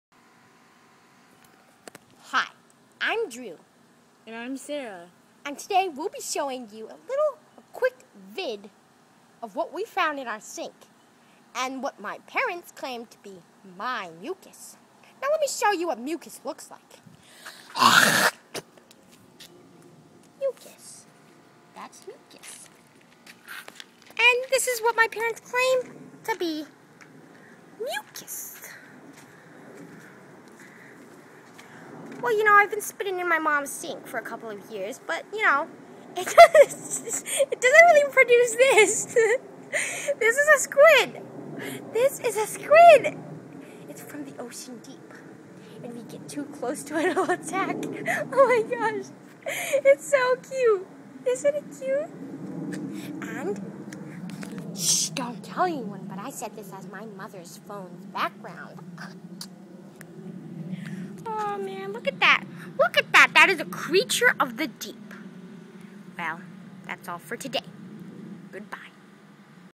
The Mucus Squid
Gross thing we found in our sink